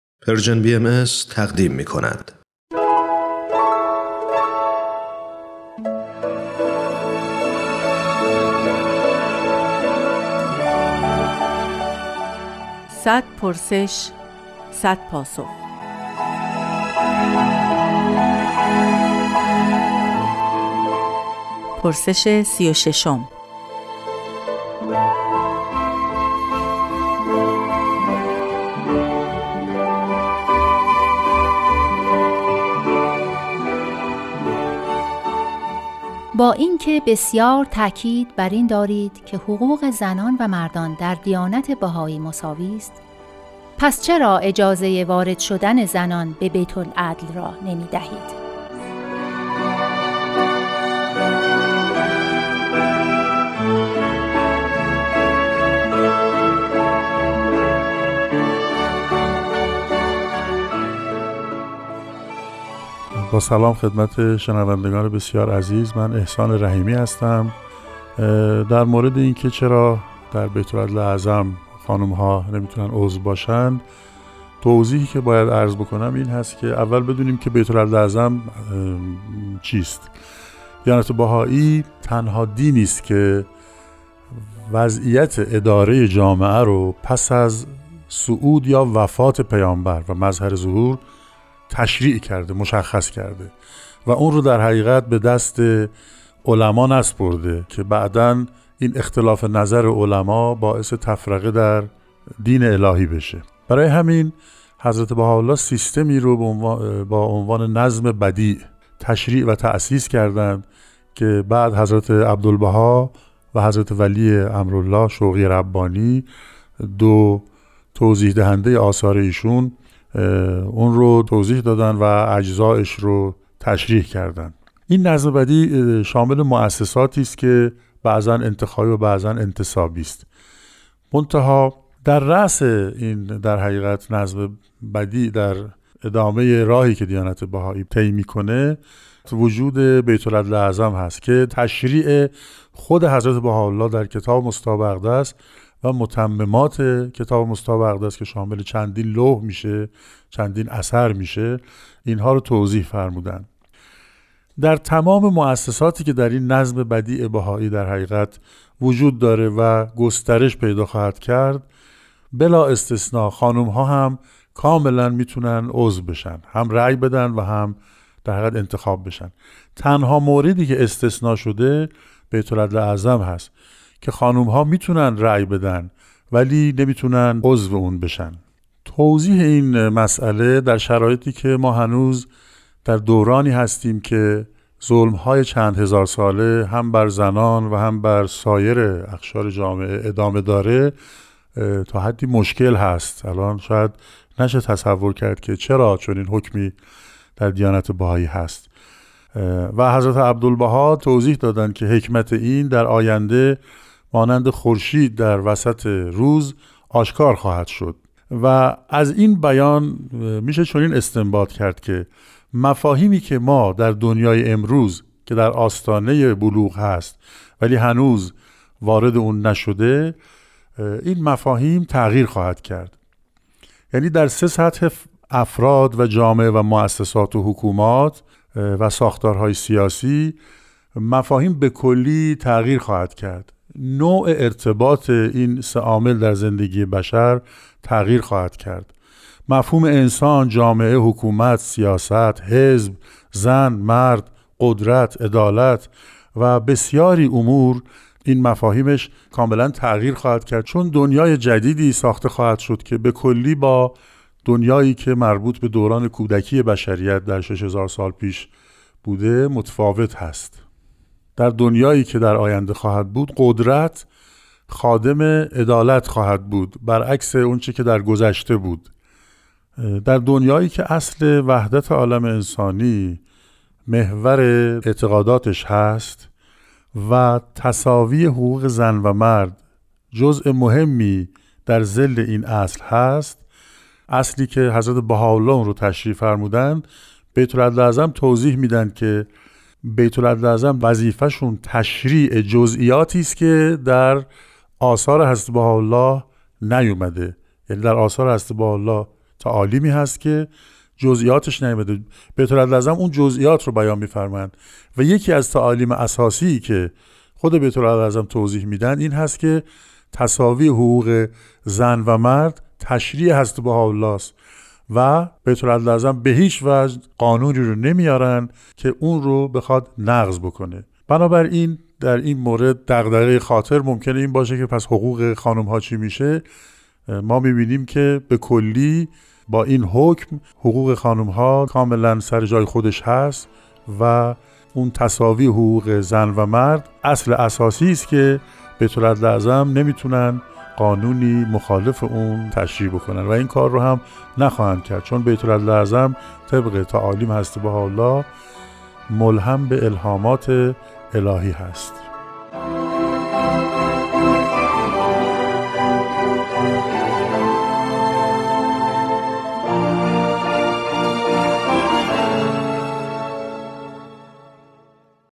پرسش ۳۶ – زنان و بیت‌العدل
با اینکه بسیار تاکید بر این دارید که حقوق زنان و مردان در دیانت بهایی مساوی است، پس چرا اجازه ی وارد شدن زنان به بیت العدل را نمی‌دهید؟